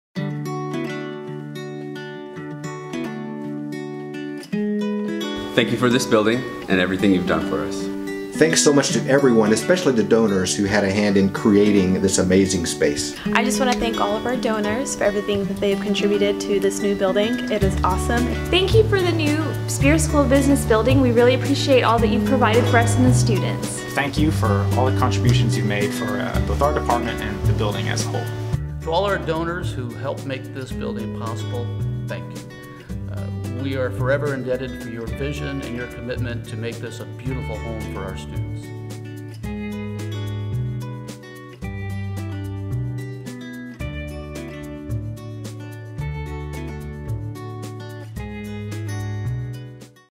Thank you, donors!
Students, faculty and staff share their thanks to the generous donors and supporters of Spear Business as they celebrate one year in the state-of-the-art building.